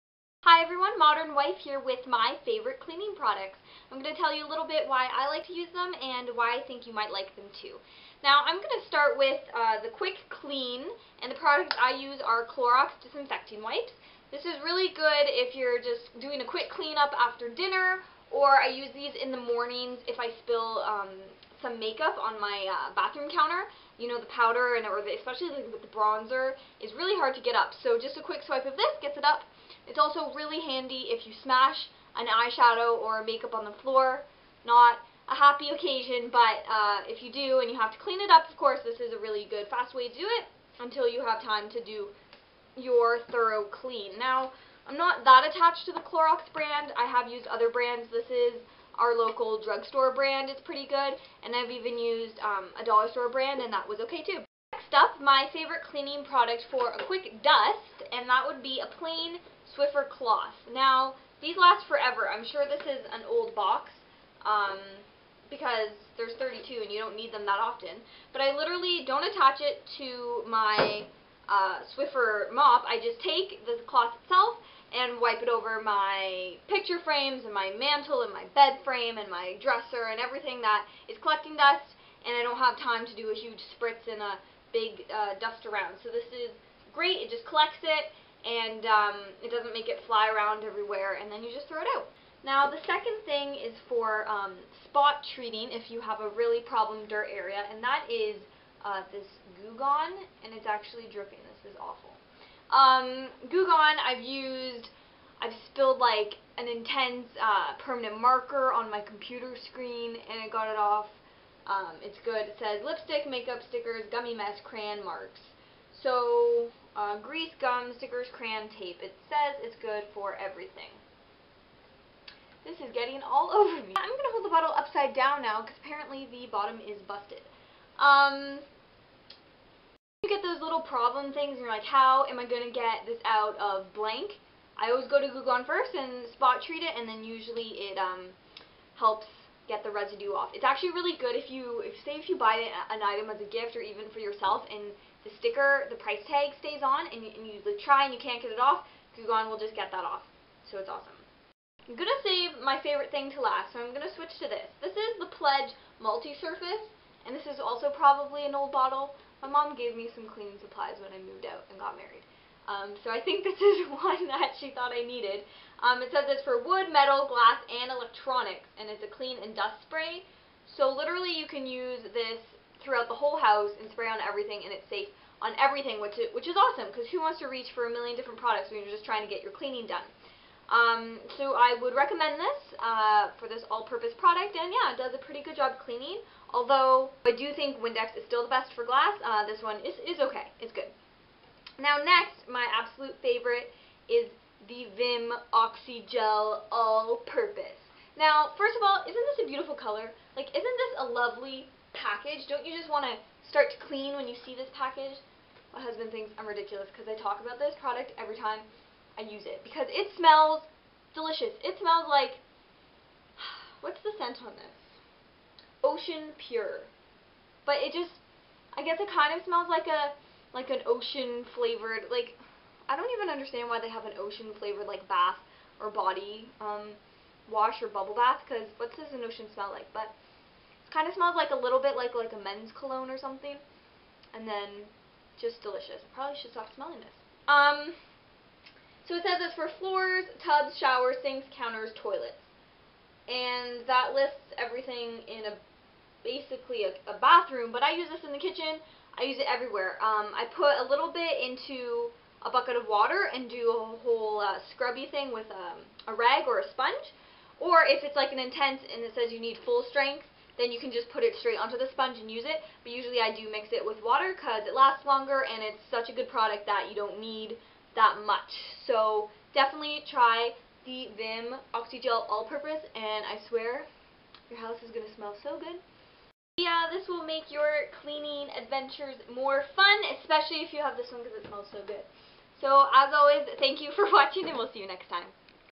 My Favorite Household Cleaning Products
I get excited about cleaning! From quick swipe to heavy duty scrub, these are my 5 favorite cleaning products!